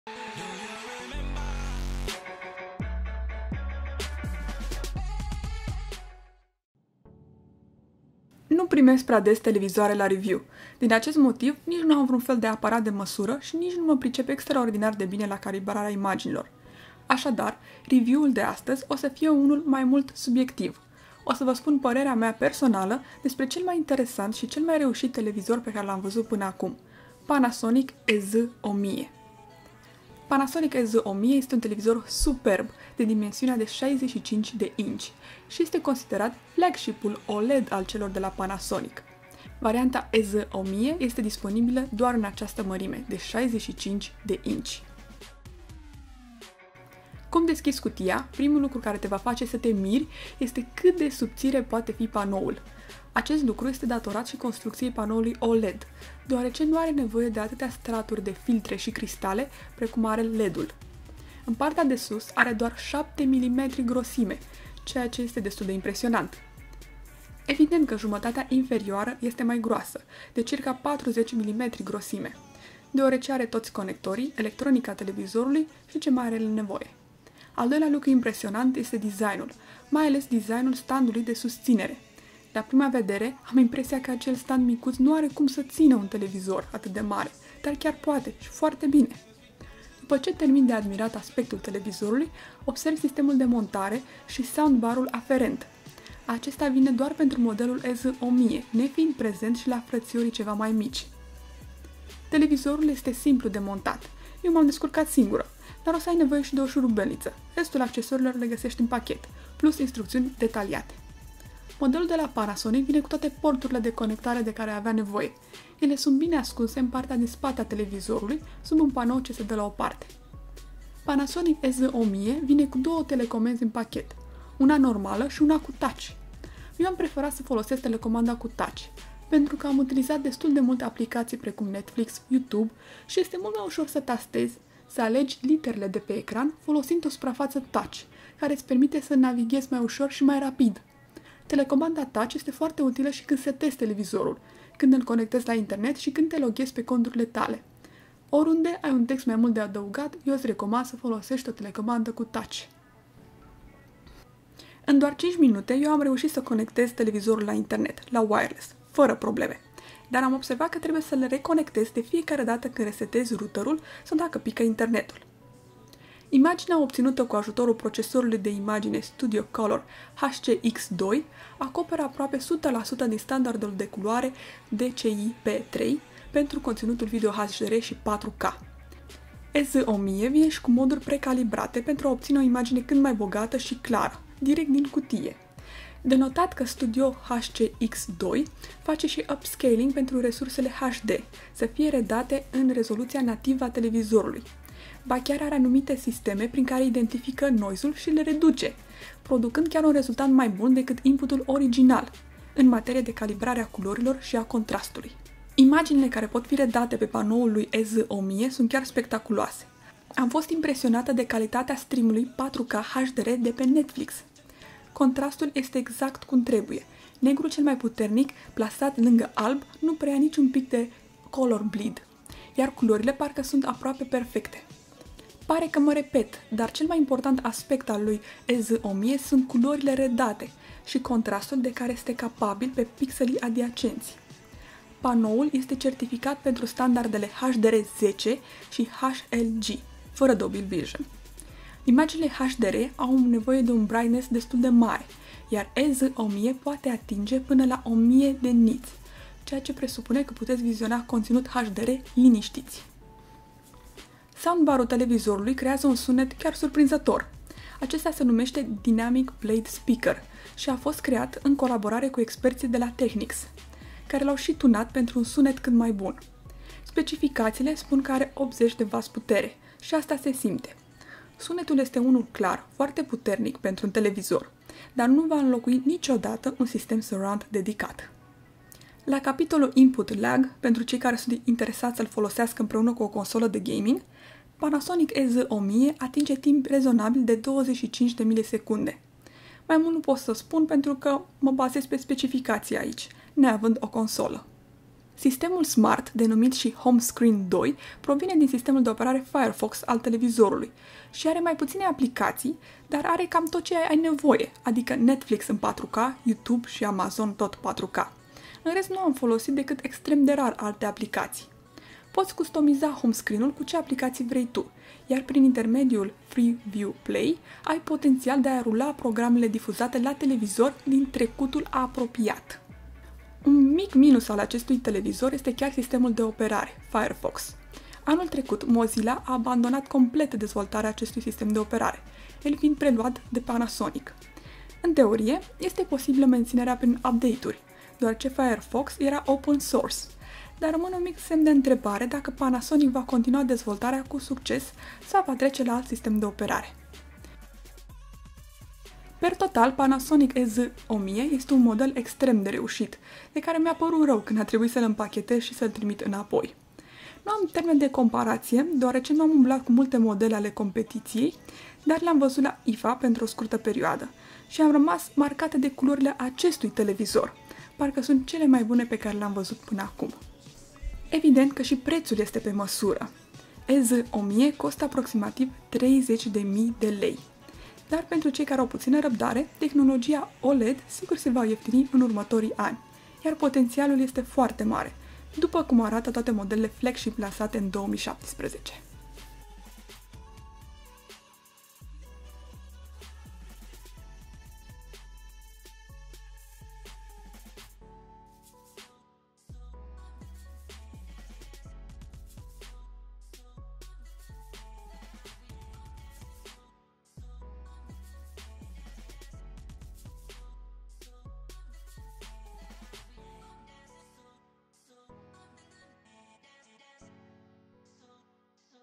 Review Panasonic EZ1000 - Un super TV OLED 4K HDR
Le-am selectat special! Spor la cumpărături!

Panasonic EZ1000 este un televizor superb de dimensiunea de 65 inci și este considerat flagship-ul OLED al celor de la Panasonic. Varianta EZ1000 este disponibilă doar în această mărime.

Foloseste acest link cand comanzi de pe eMag.ro daca vrei sa ma ajuti sa realizez materiale de o calitate mai buna. (Link-ul este unul de afiliere.)

Un nou video săptămâna viitoare!